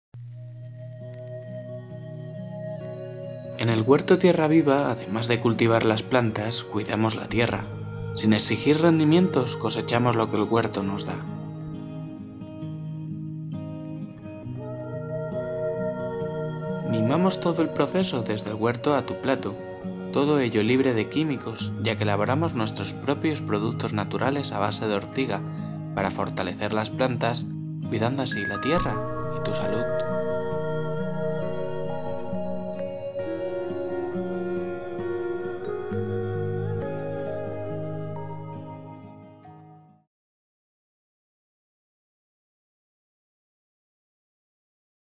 Spot Huerto TierraViva 1 - José Villaescusa
En el huerto TierraViva se cultiva ecologicamente sin usar ningún tipo de químico, dando fuerza y vigor a las plantas, gracias a elementos naturales como la ortiga y al gran trabajo de profesionales siempre a tu disposición.

Si quieres realizar un pedido puedes hacerlo a través del mail

Busca más información en

Blog: HuertoTierraViva.Blogspot.com    --- Productos y precios aquí

Vídeo Realizado por José Villaescusa y Diego Delgado Elguera
Cámara Diego Delgado Elguera y José Villaescusa
Edición y Etalonaje José Villaescusa

Se han usado los siguientes programas:
After Effects
Premiere Pro
Audition